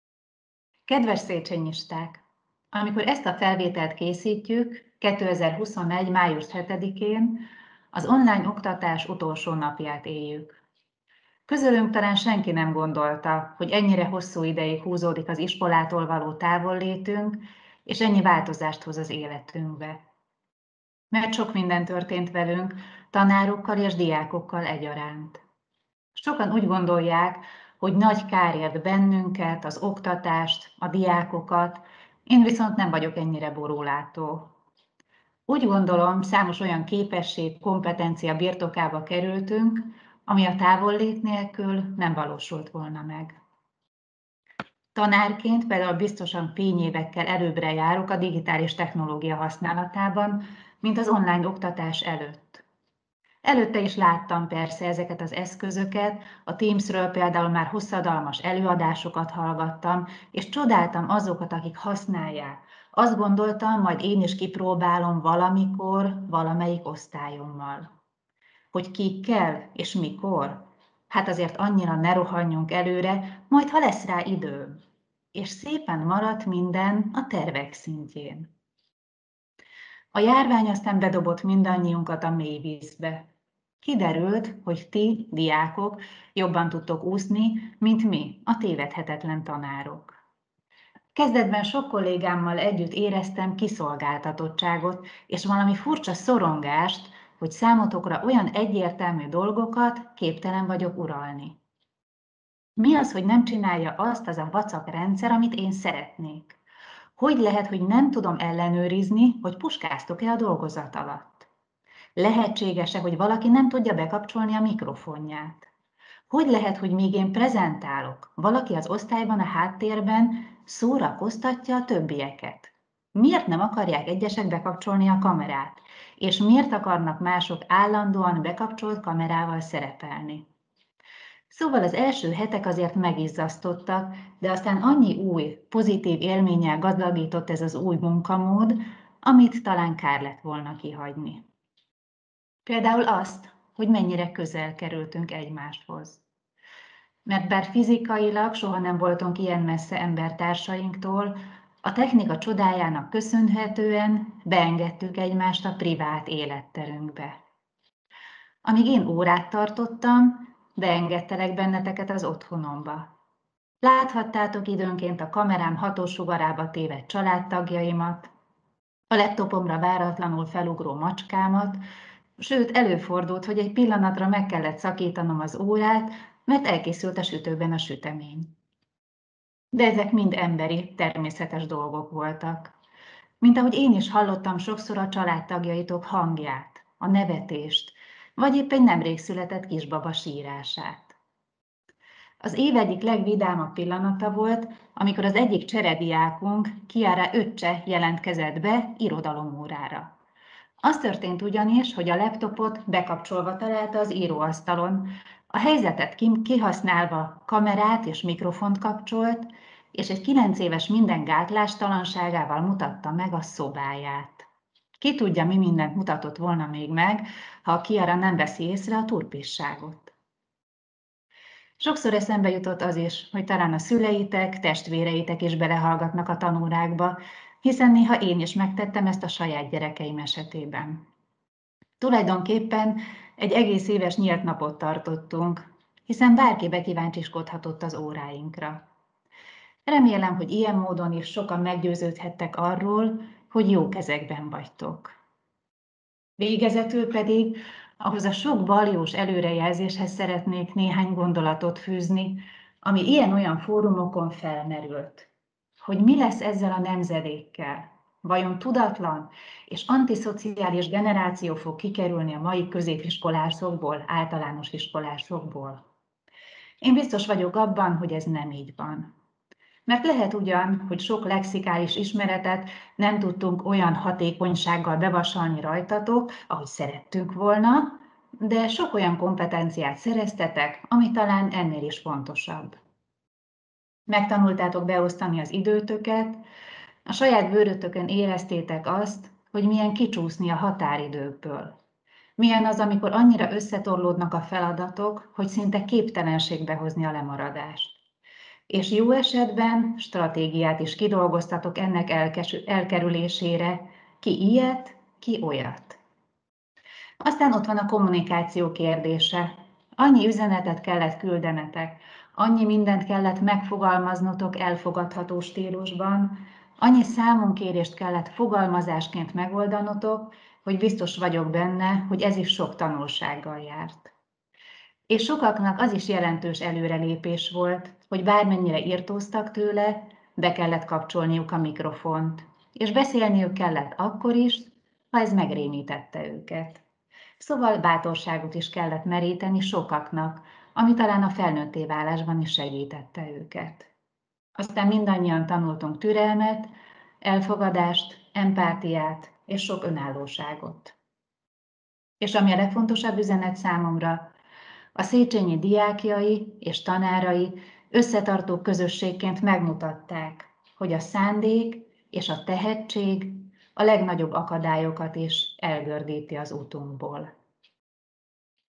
Egy időszak vége (Juhászné Sebestyén Andrea tanárnő) - A hét gondolata #5
A jelenléti oktatás újraindulásával itt az idő átgondolni, mit nyertünk a nehéz időszakkal.